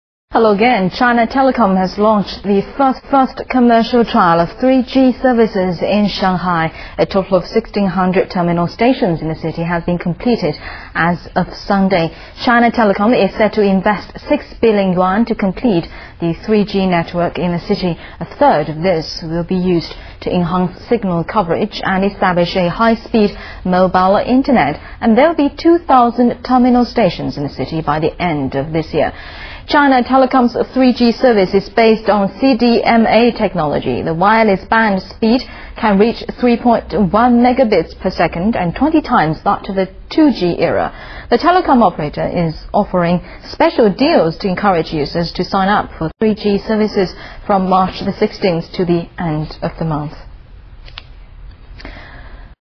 China Telecom launches commercial 3G trial services
China Telecom has launched the first commercial trial of 3G services in Shanghai. A total of 1600 terminal stations in the city had been completed as of Sunday.